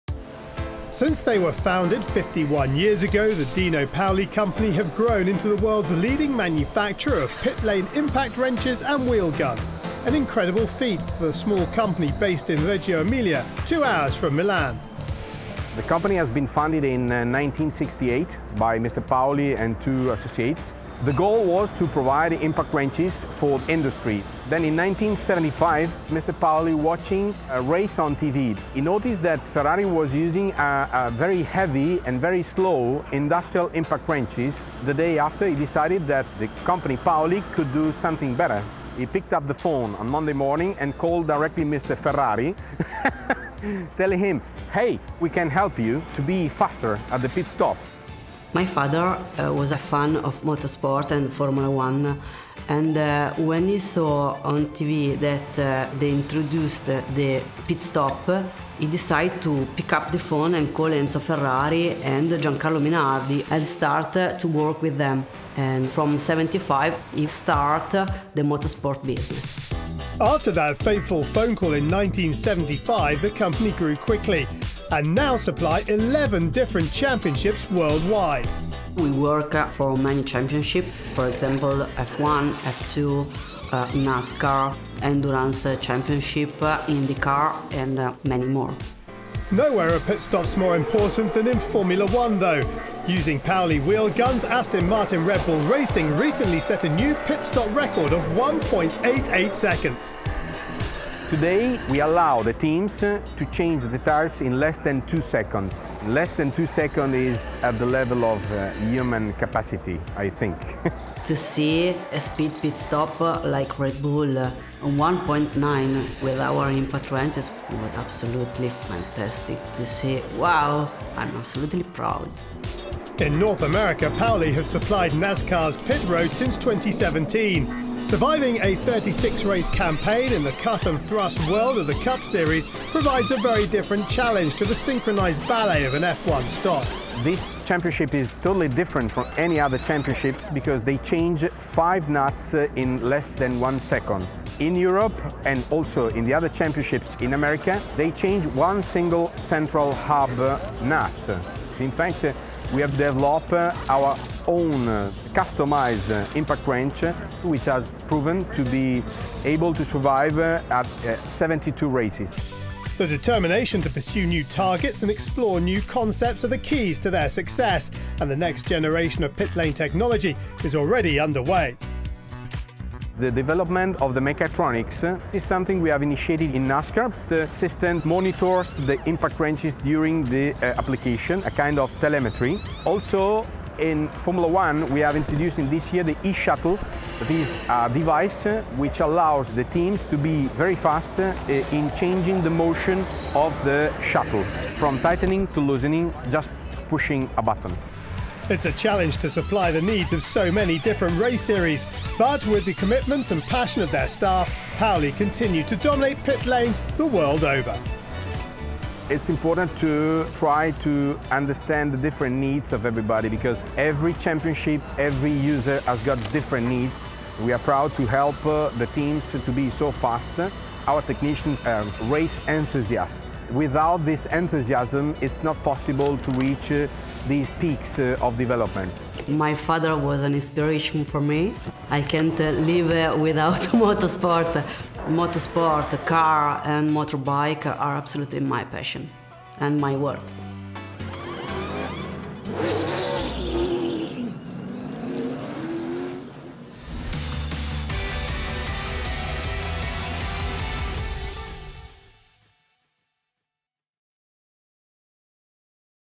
Sudden Impact – How Dino Paoli Wheel Guns Revolutionised The Pit Stop | M1TG
Mobil 1 The Grid heads to the Dino Paoli impact wrench factory in Italy to find out how a phone call to Enzo Ferrari led to one company providing wheel guns to all the major race series. The Formula 1 pit stop record fell to 1.88 seconds with Aston Martin Red Bull Racing's stop at this year's German Grand Prix and Dino Paoli continue to innovate in F1, NASCAR and other championships.